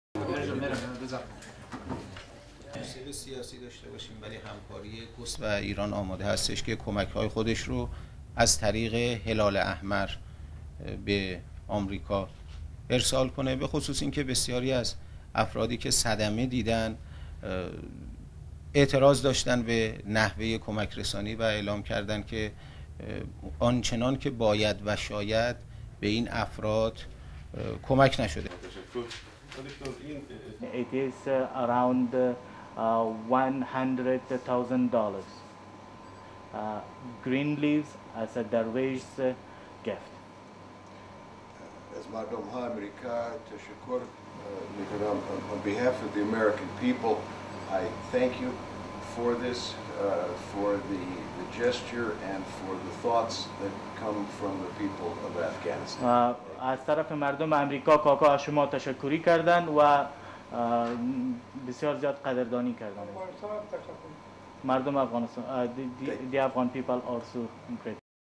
WRAP Afghanistan and Iran offer aid for Katrina victims
Tehran
1. Hamid Reza Asefi, spokesman for Iranian Foreign Ministry, enters news conference
2. Journalists
3. SOUNDBITE: (Farsi) Hamid Reza Asefi,Spokesman for Iranian Foreign Ministry
"Iran is ready to send its humanitarian aid to America via the Red Crescent especially when many of the needy survivors of the hurricane were not satisfied with the process of aid and believed that they were not helped as much as they deserved."
4. Wide of news conference

Kabul
5. Afghan representative at US Embassy, Khan Mohammad handing over document to US Ambassador Ronald Neumann.
6. UPSOUND (English) Ronald Neumann, US Ambassador
(Translator:  "It is around 100-thousand dollars".)
"On behalf of the American people I thank you for this gesture and for the thoughts that come from the people of Afghanistan."

STORYLINE:

Iran said on Sunday it was ready to send humanitarian aid to the victims of Hurricane Katrina.

Speaking in Tehran on Sunday Iranian Foreign Ministry spokesman Hamid Reza Asefi said the aid would be sent via the Red Crescent.

Asefi said survivors of the disaster "believed that they were not helped as much as they deserved."

A decades long rift between Iran and the US was re-fuelled last month after Iran resumed its uranium reprocessing activities.

Iran claims the nuclear program is for civilian use only, but the United States accuses Iran of using the program as a cover to secretly produce nuclear weapons.

Meanwhile US Health and Human Services Secretary Michael Leavitt said on Sunday the death toll from Hurricane Katrina and its aftermath is in the thousands, the first time a federal official has acknowledged what many have feared.

Leavitt said he could not provide a precise number on the impact of the devastation, but when asked if it was in the thousands, he told CNN''s "Late Edition," "I think it''s evident it''s in the thousands."

Also on Sunday, at the US embassy in Kabul, the Afghani government handed over aid support worth 100-thousand US dollars (euro 79-thousand) to help victims of the hurricane.

US Ambassador Ronald Neumann said the gesture from the cash-strapped government was highly appreciated.

Afghanistan relies heavily on financial aid from the United States and elsewhere.

The United States has asked the European Union and the NATO alliance for emergency assistance, requesting blankets, first aid kits, water trucks and food for the victims of Hurricane Katrina, the two organisations said on Sunday.

So far, Austria, Belgium, Britain, Denmark, Estonia, France, Germany, Greece, Luxembourg, Finland, Italy, Slovakia, Slovenia Sweden, Spain and The Netherlands have pledged aid.